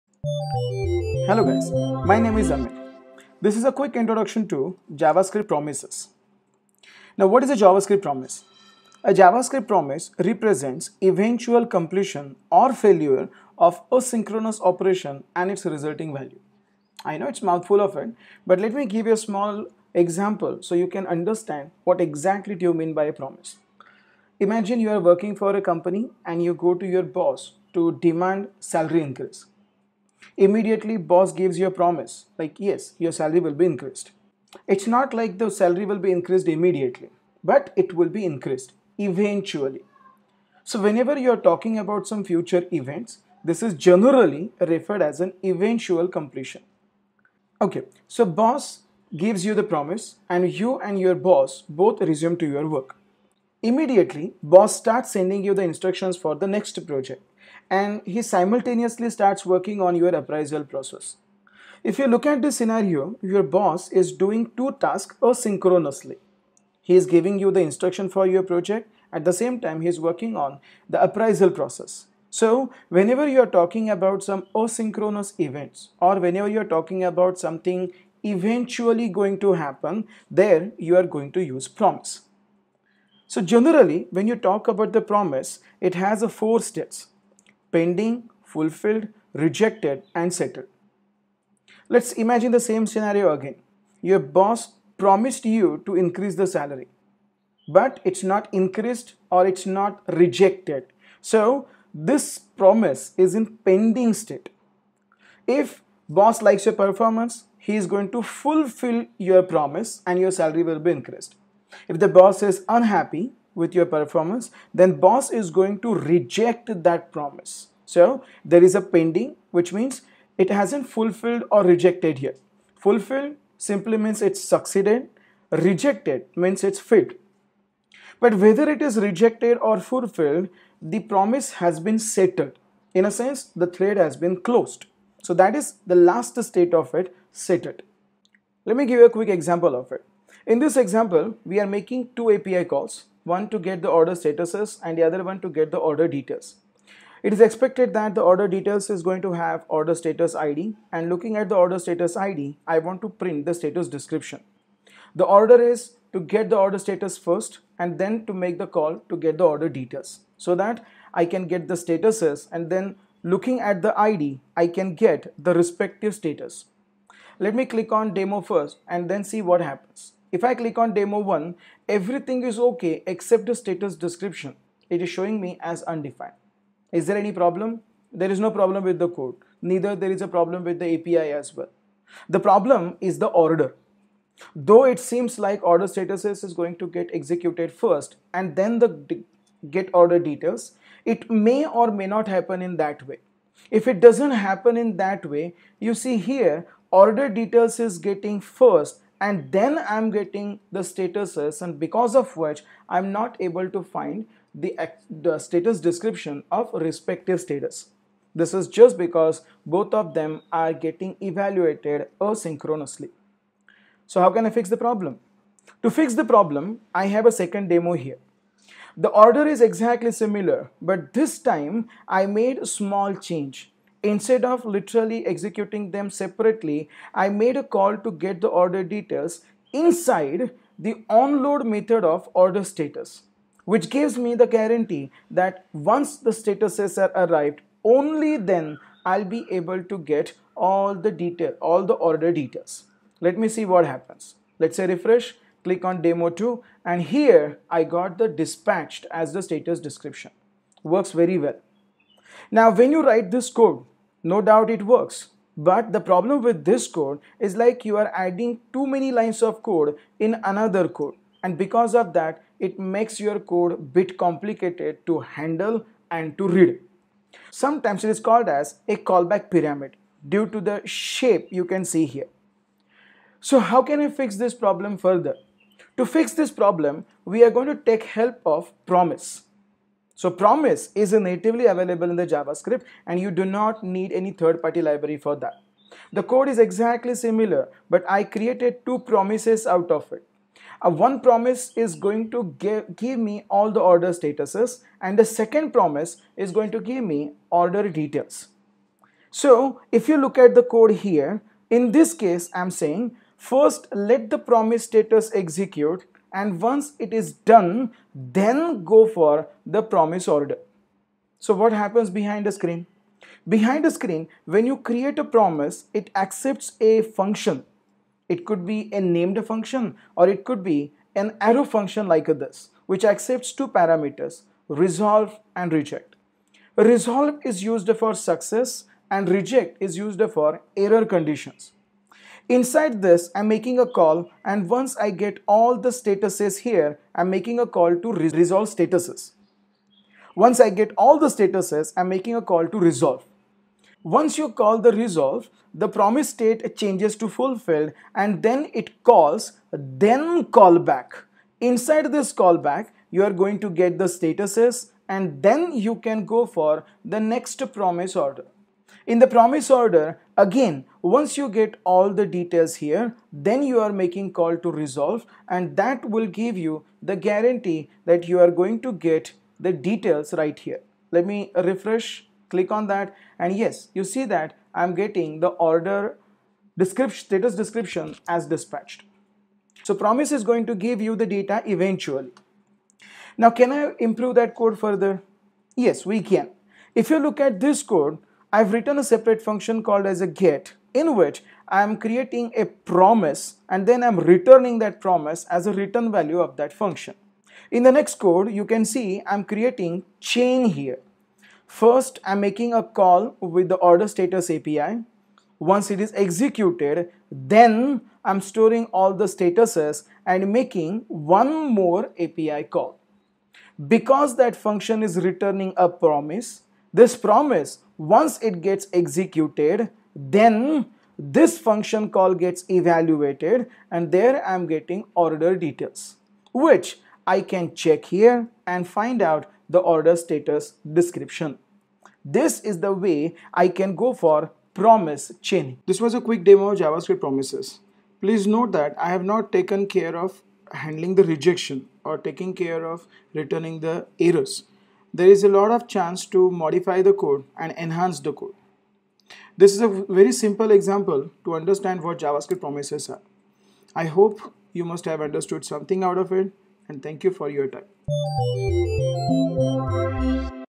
JavaScript Promises
This is a small demo of JS promises.